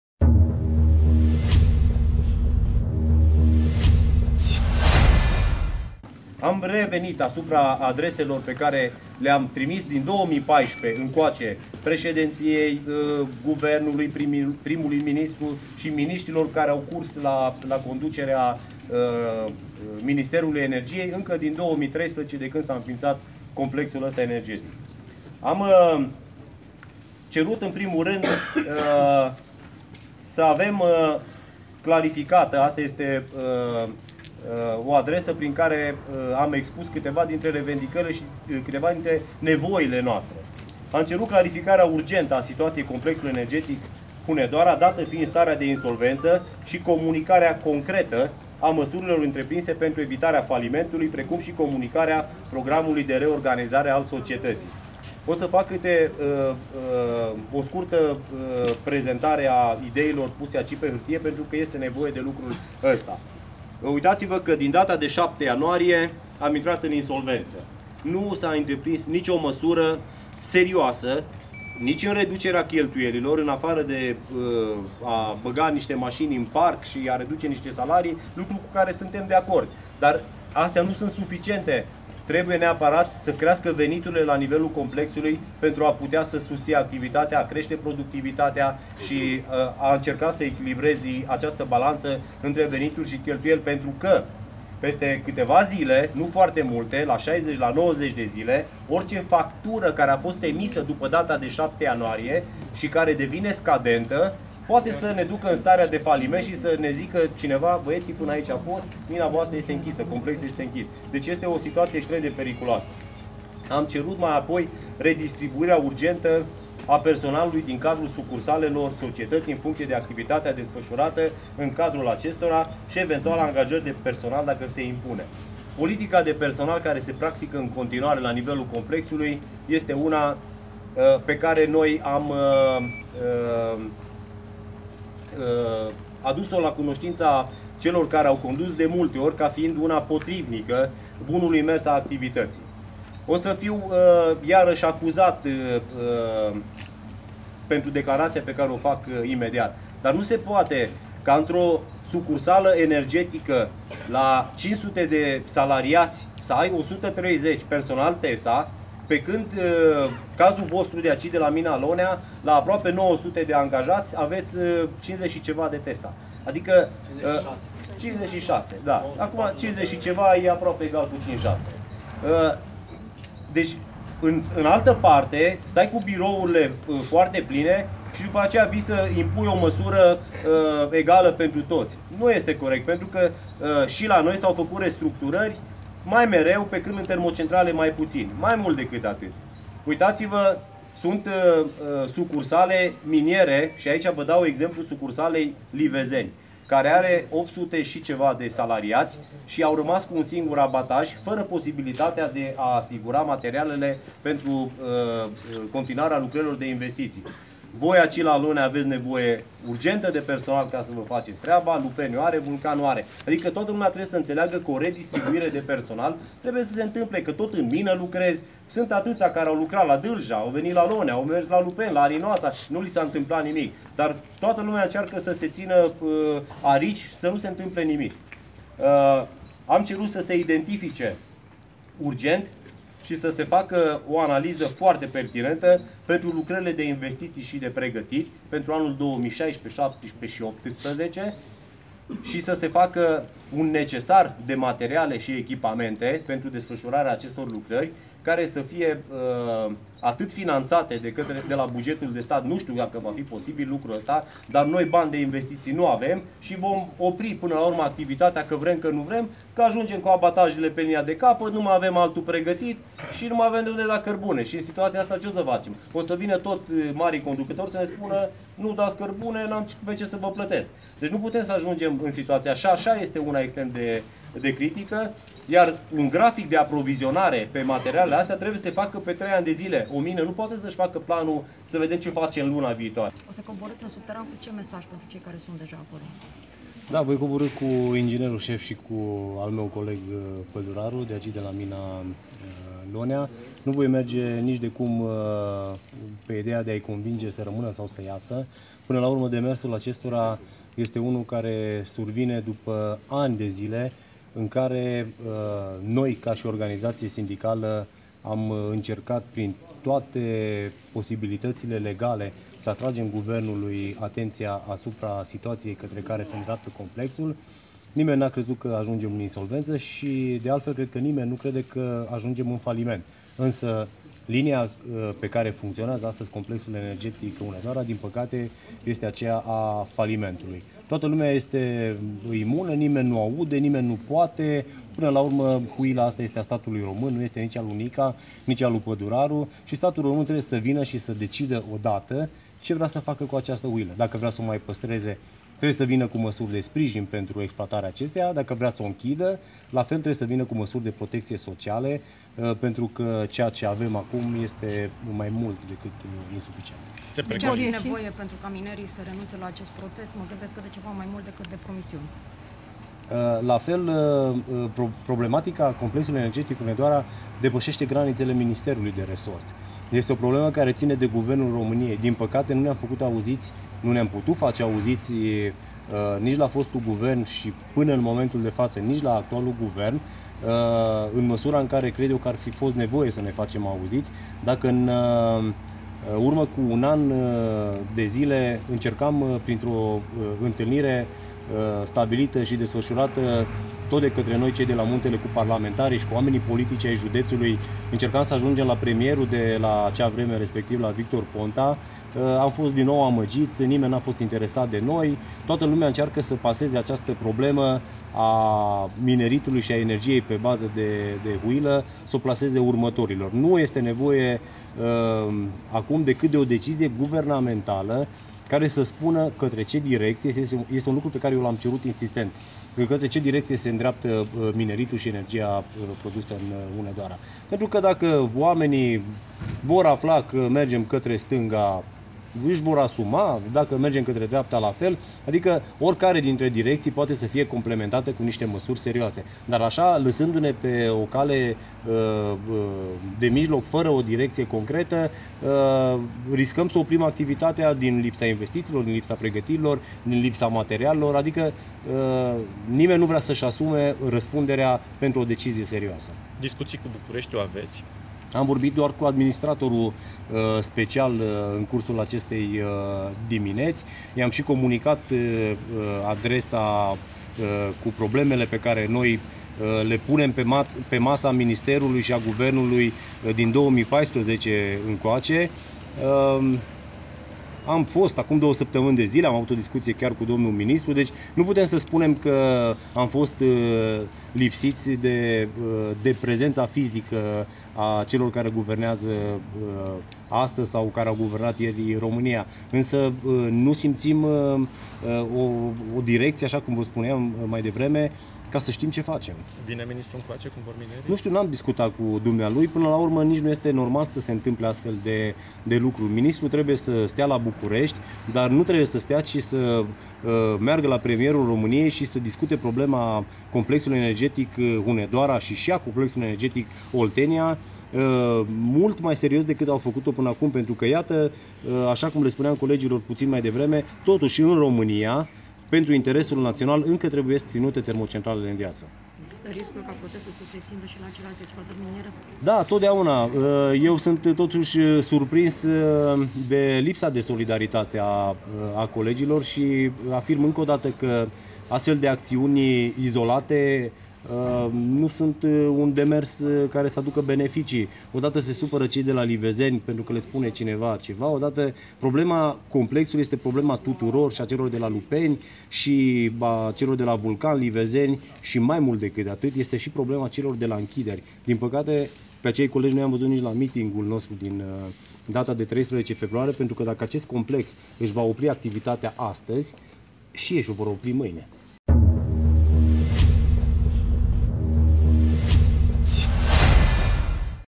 HUNEDOARA - Mina Lonea
Liderii Sindicatului 'Muntele' de la Complexul Energetic Hunedoara (CEH) au coborât luni dupã amiazã în subteranul Minei Lonea pentru a discuta acolo cu minerii care s-au autoblocat, în cursul dimineţii, la locul de muncã, nemulţumiţi de situaţia economicã în care se aflã societatea.

Imagine: Sorin Blada
Editor: Mihai Georgescu